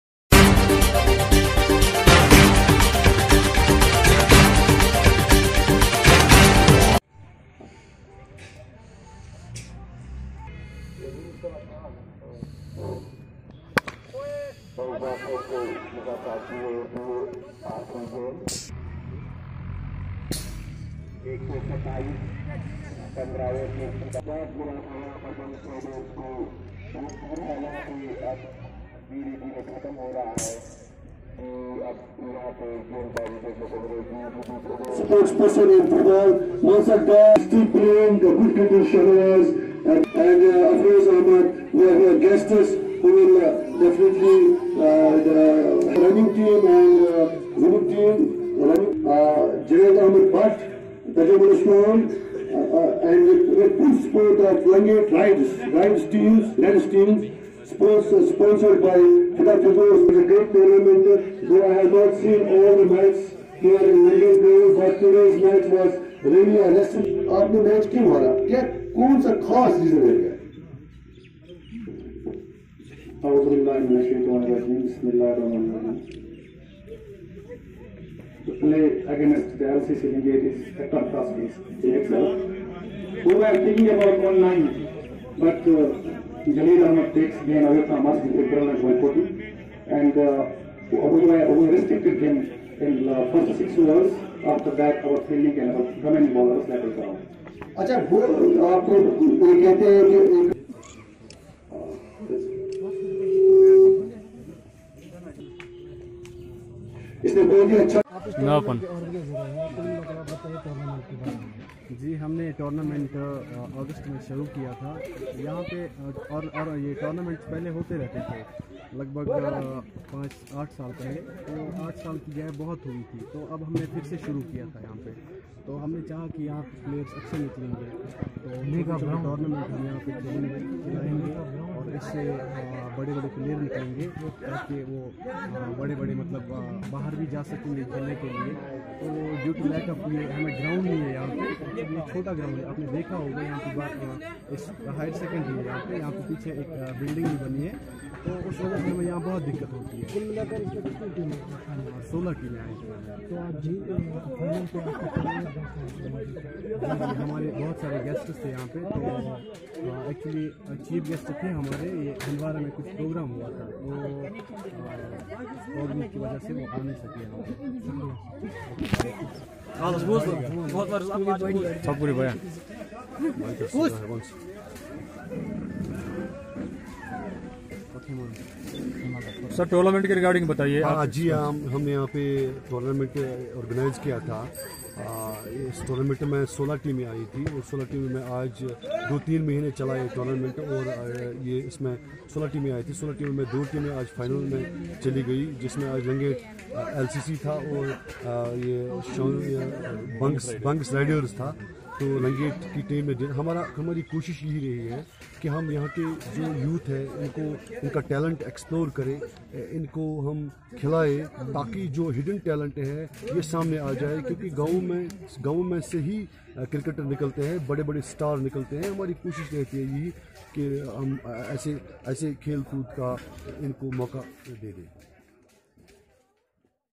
Champions trophy lngate was organised by javid ahmad bhat tajamul islam mir in govt girls hghr sec s
Champions trophy lngate was organised by javid ahmad bhat tajamul islam mir in govt girls hghr sec school lngate total no.of teams who participated in tornmnt wre 16 no. Today final of tbe tornmnt was played btween lcc lngate vs bungus riders shanuu lcc won the toss nd made 241 runs wth four wckts down jaleel ahmad played best nd made unbeaten 140 nd remained ntout hlps his team to a good total 241
Whreas bungus riders shanuu played return bck the open spell by showkt ahmad of lcc got fst wckt in fst over hlps his team to got a best breakthrough whreas  the middl ordr bats man  played well nd pick good start made a best partnrship of 60 runs in 5 overs nd thn dilawar of lcc got anothr breakthrough hlps his team to tke a showoff winning side while as shanuu riders wre hoped to win the batsman of shanuu wre fall down early wickts and results they lost

Presntation
Guests  ishaq ahmad dar   azad ahmad bhat mohd iqbal qurrshi firdous ahmad bhat mushtaq ahmad lone afroz ahmad lone wre the guests of the tournmnt with media persons  a trophy of sub gold wre given to winner team nd a cash prize of award wre also given to both teams bilal ahmad lone was best catcher of tournmnt nd mohd dilawar mir was man of the series jaleel ahmad was man of the match